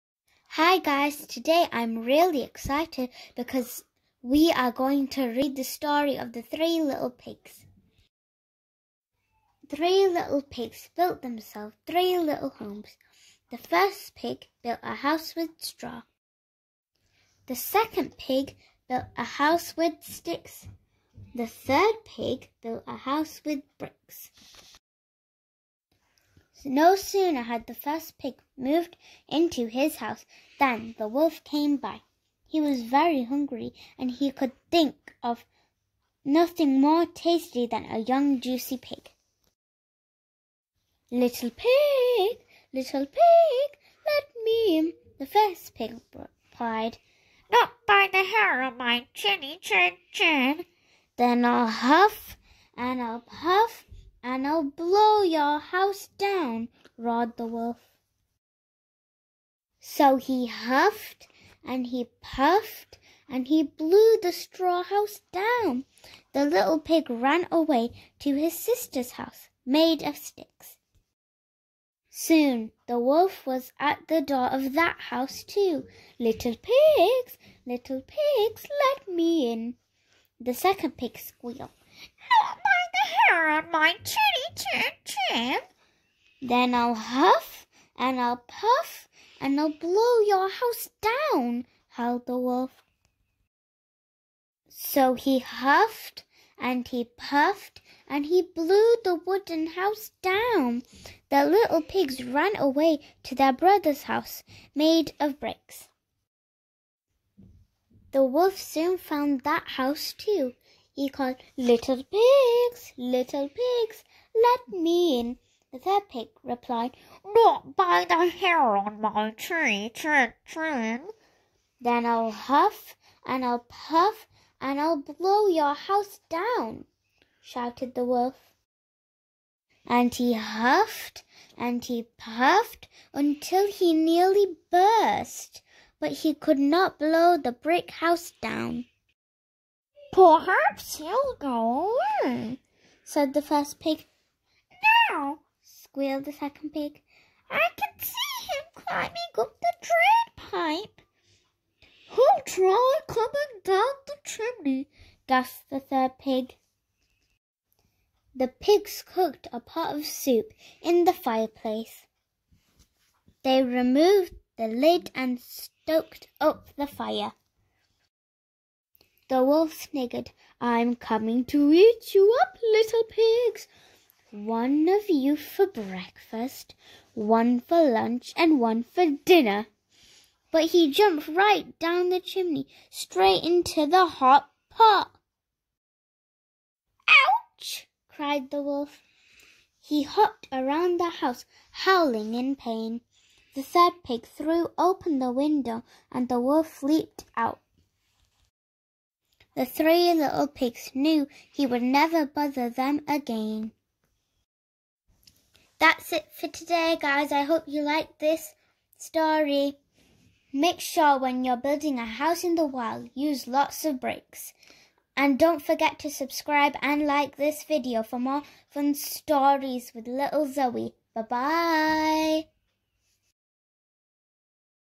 Storytelling for kids | Educational | fun | Zoey's story time | The Three Little Pigs
three little pigs built themselves three little homes but a hungry wolf attacked them
would they be able to get rid of him ?